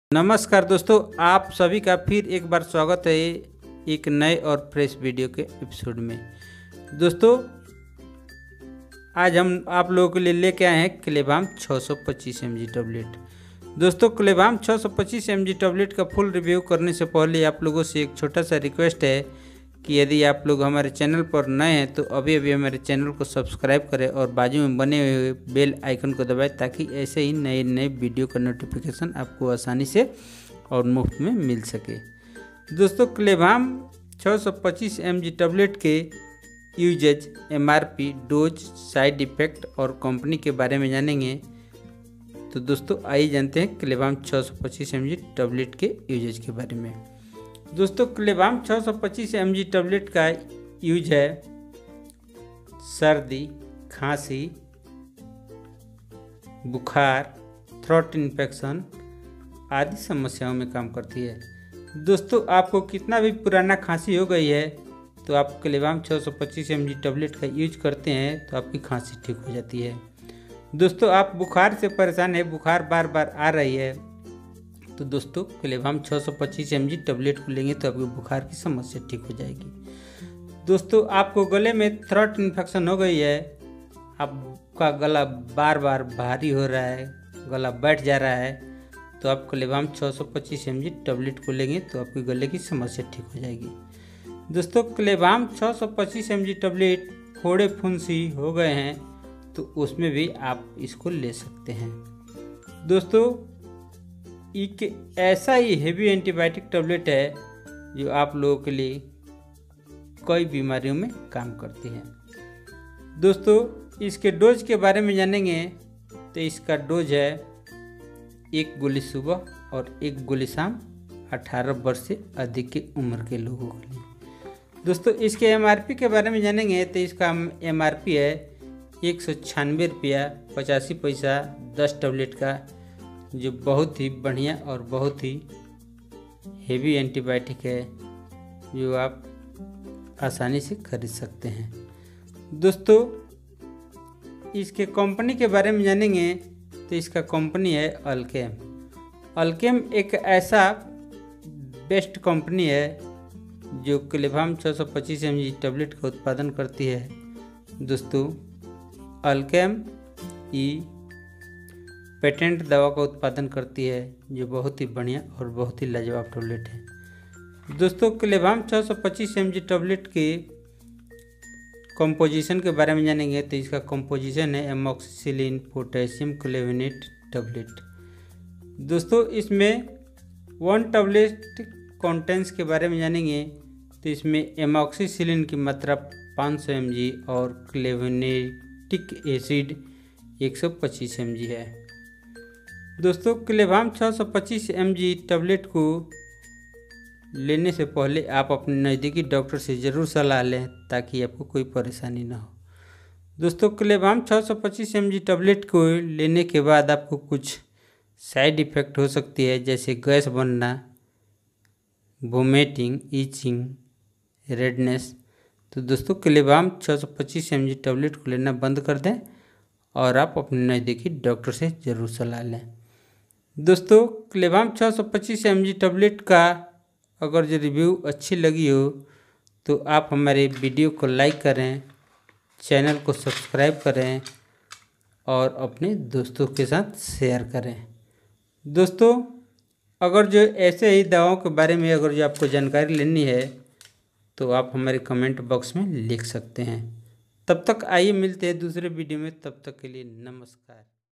CLAVAM 625 mg tablet ka full review ,does mrp side effects HS MEDICO
namskar dosto aap sabhi ka swgat hai hamare you tube channel HS medico me ,

clavam 625 mg tab ka mrp hai 198 rupaye,

clavam 625 tab ka composition hai Amoxicilin potassium clavunate,

clavam 625 mg tablet ka does hai 18 year above 1 tablet 2 time ,

clavam 625 mg tablet ka uses hai caugh ,khansi ,fiver , bacterial infection , throat infection phode phunsi etc .